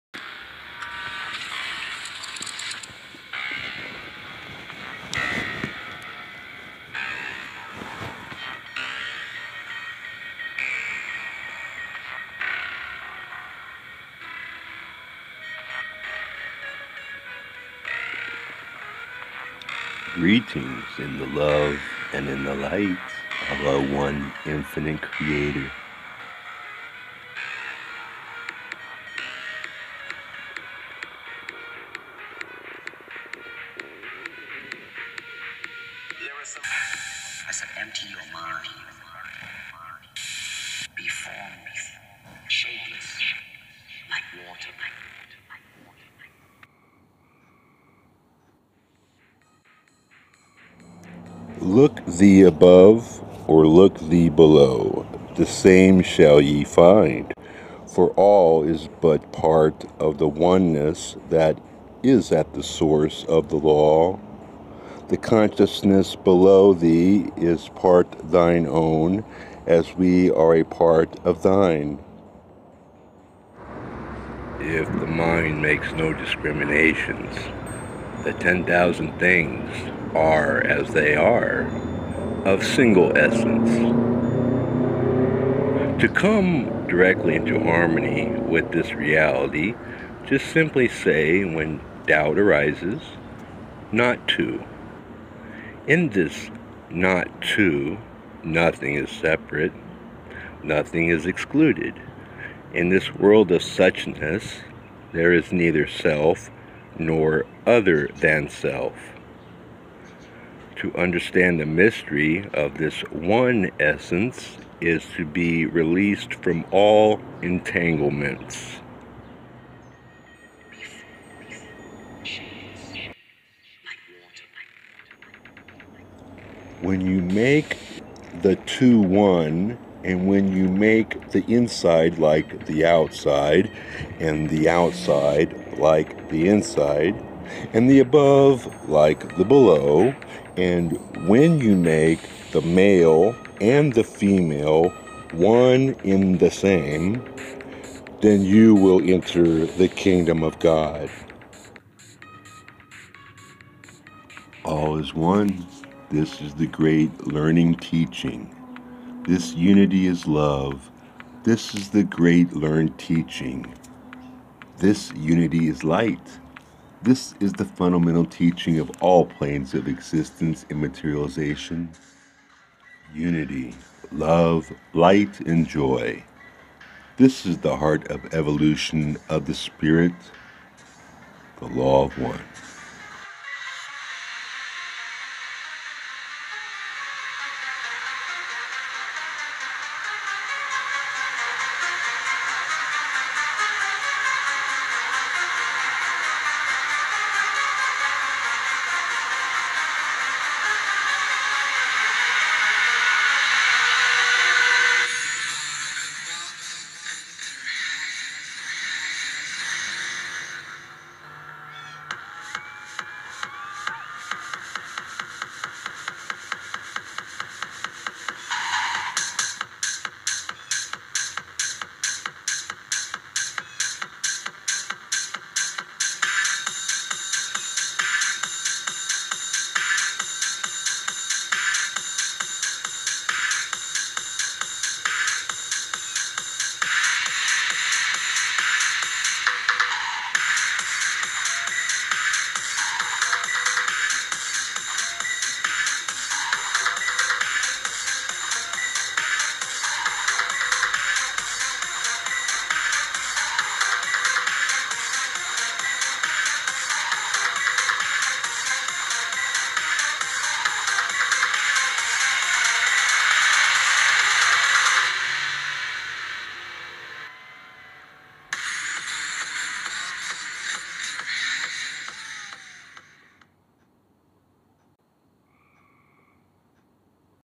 Seeing All As One In The Creator's Unity. The Law Of One
Teachings of Oneness of all the Creation.

Quotes from:

-The Emerald Tablets
-Hsin Hsin Ming
-Jesus
-Ra

In love and light video and narration by an humble messenger of
The Law of One- Robert